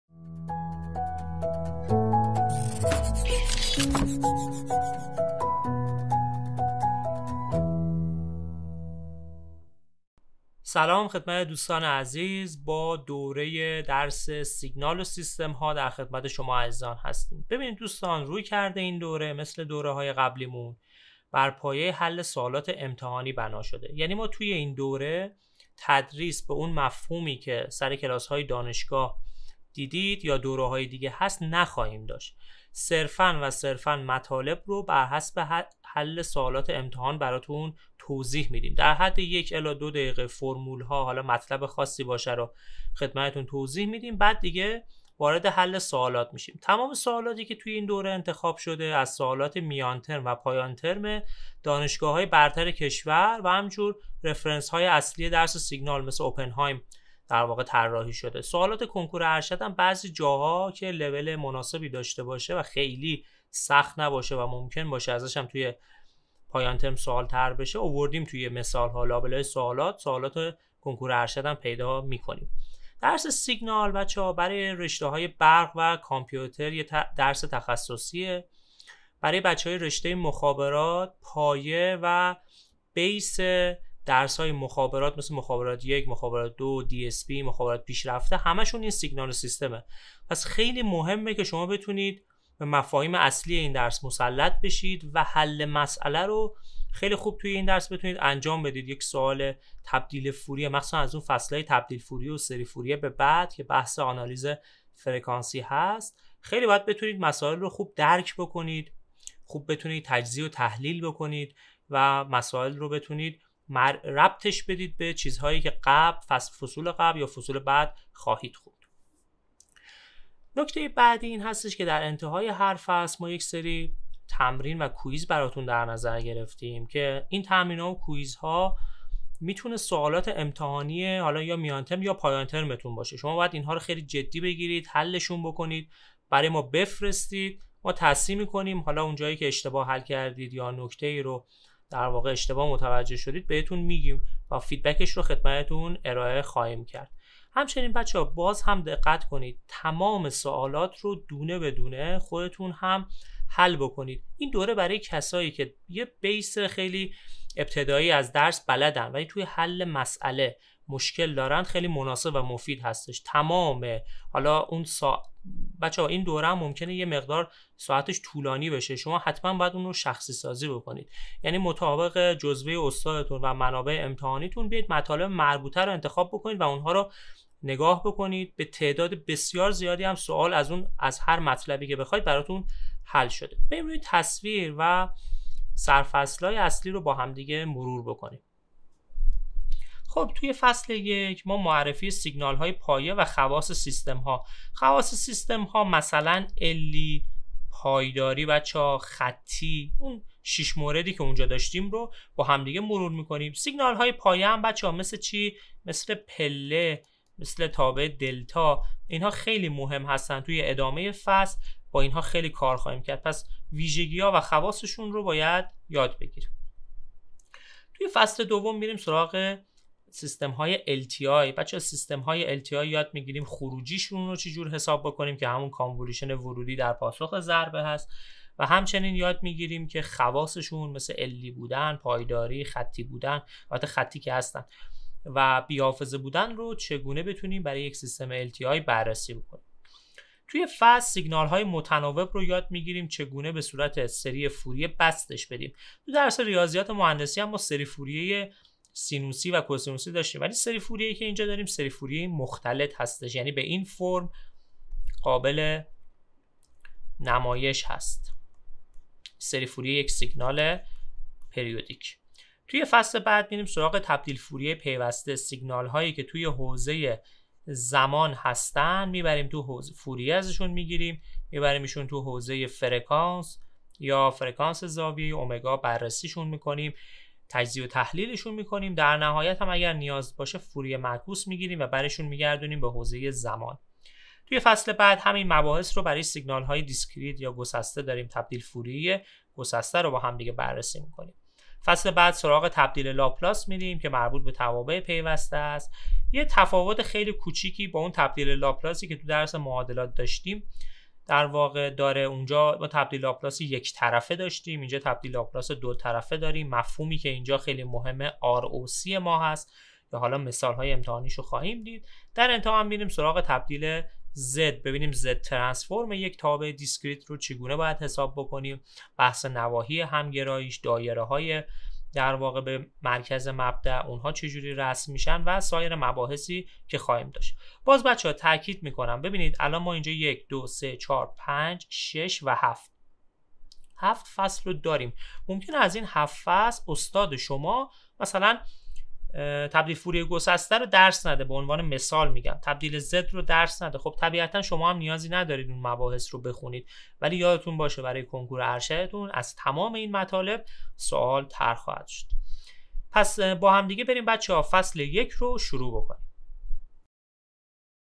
آمادگی امتحان سیگنال و سیستم ها قسمت ۱
این دوره‌ی آمادگی امتحان سیگنال و سیستم ها از سری آمادگی شب‌های امتحان می‌باشد. رویکرد این دوره بر پایه‌ی حل سوالات امتحانی است. تمام سوالاتی که در طول این دوره حل می‌شود از سوالات امتحانات پایانترم و میانترم دانشگاه های برتر کشور انتخاب شده‌است.

استاد: عرفان صابر

برای مشاهده  آمادگی امتحان سیگنال و سیستم‌ها در مکتب‌خونه از طریق لینک‌ زیر وارد شوید